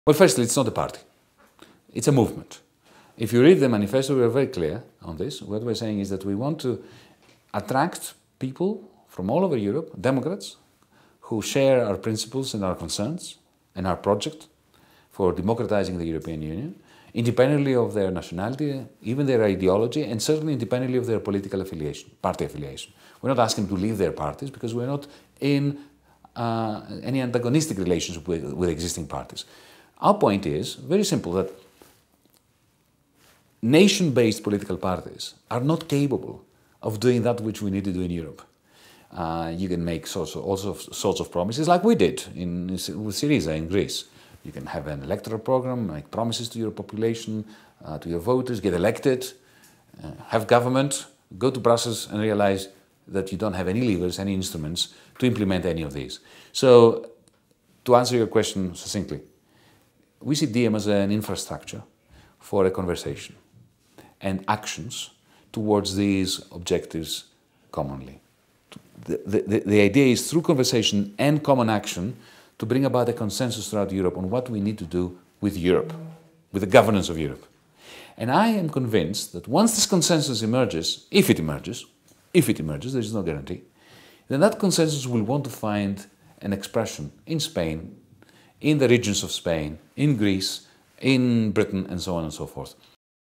Varoufakis: "Los partidos nacionales no pueden democratizar Europa"
Fecha: 19/02/16

Redacción: Alejandro López de Miguel /@Alopezdemiguel
Imagen: La Barraca Producciones